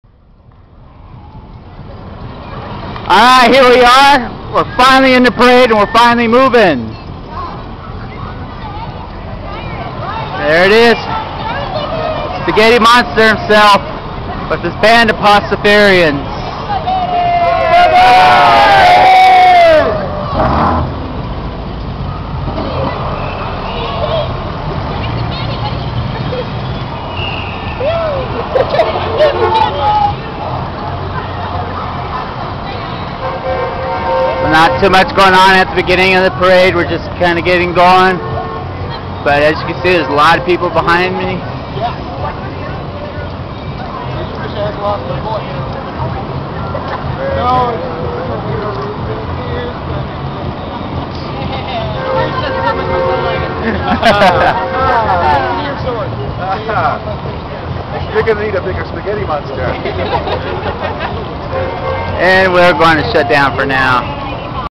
Seattle Atheists, FSM, Seattle Gay Pride Parade 2008 1 of 3
Seattle Atheists Marching in the Seattle Gay Pride Parade of 2008.

For more information about the Seattle Atheists please go to our website at: